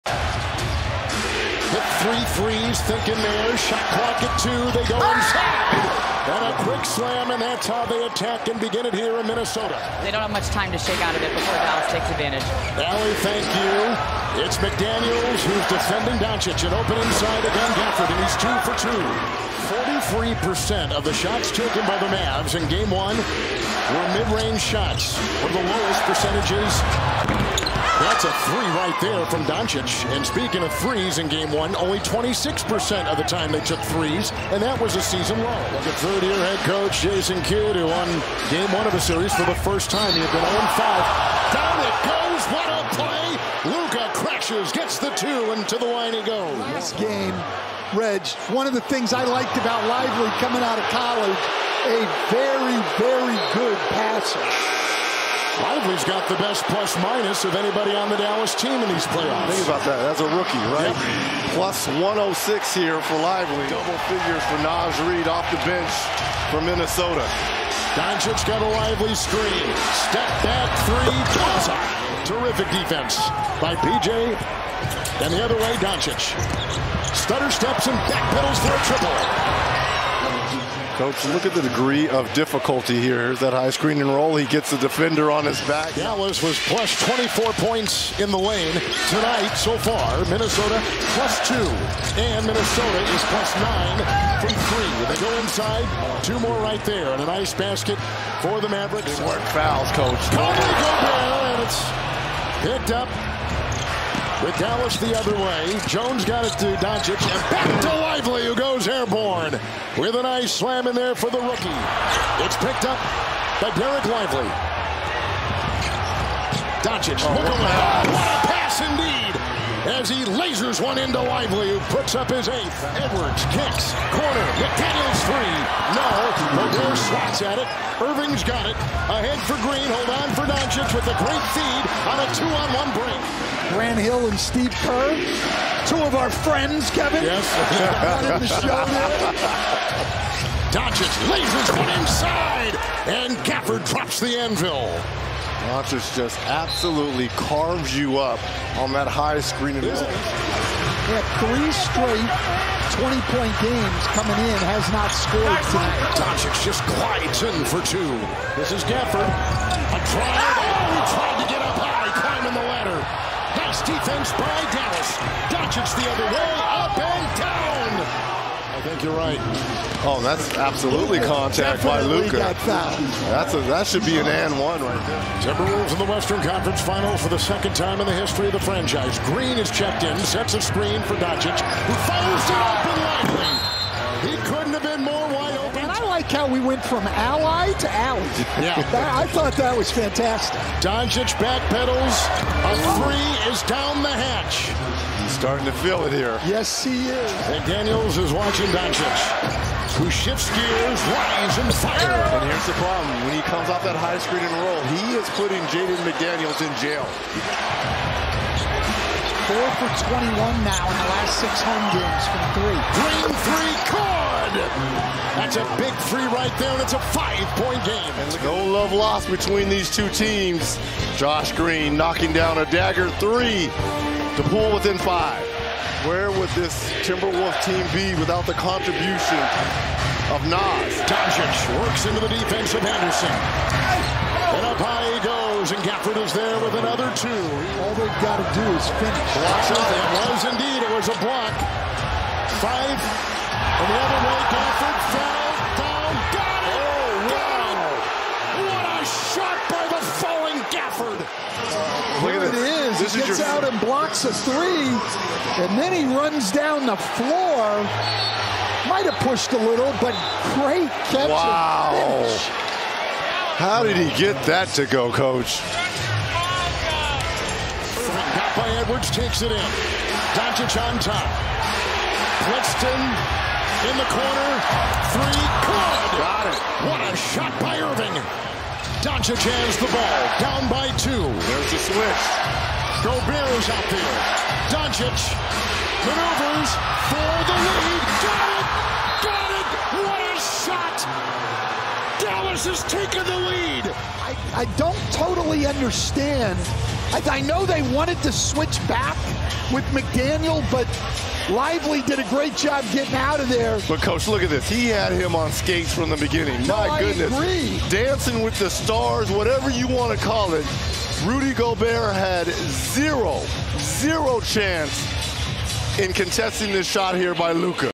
Luka Doncic Insanely Clutch Game 2 Performance PLAYOFFMODE | May 24, 202
Luka Doncic made a step back 3PT FG with 00:03 seconds left on the clock and the Wolves could not convert on their final possession as the Dallas Mavericks defeated the Minnesota Timberwolves, 109-108 to take a 2-0 series lead in the Western Conference Finals. Luka Doncic led Dallas with his eighth career Playoff triple double as he tallied 32 points, 13 assists, and 10 rebounds.

Game 3 of the Western Conference Finals will take place on Sunday, May 26 (8:00 p.m. ET, TNT/TruTV).